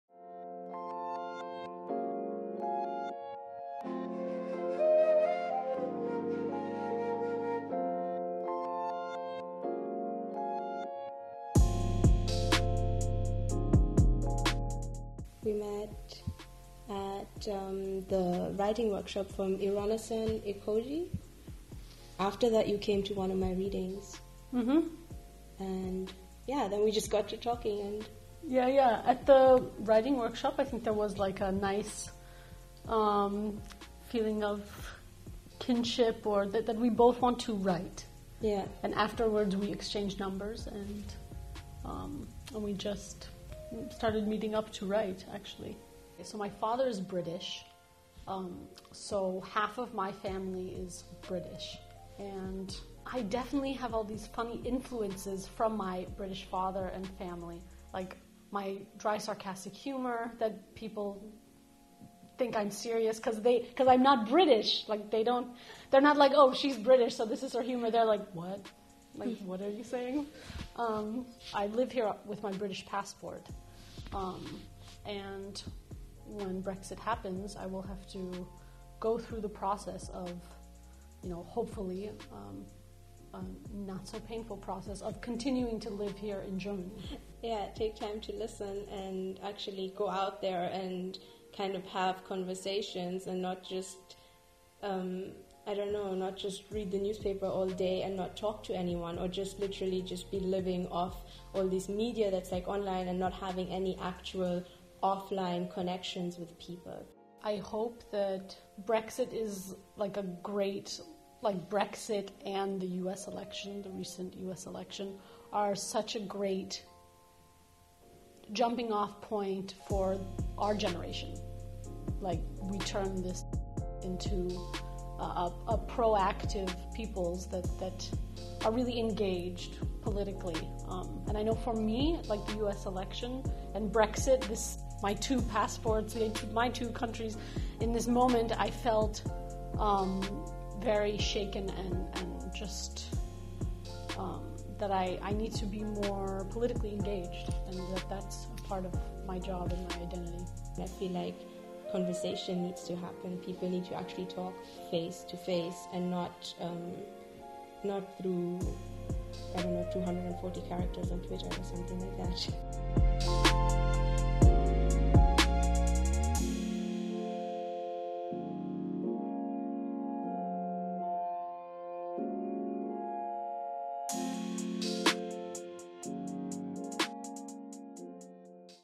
UKGermany2018 Friendship Stories: Esther & Natalya
UKGermany2018 is all about inspiring young people and providing them with inclusive platforms and opportunities. Today we are sharing Esther and Natalya's story. Esther, an Afro-German writer was chosen to be UKGermany2018's artist in residence. She met Natalya, an American-English journalist in the making, at a writing workshop and it was their shared passion for literature that brought them together. Despite both young women having totally different backgrounds, they still have a lot in common. Watch this lovely video portrait about their hopes for the future.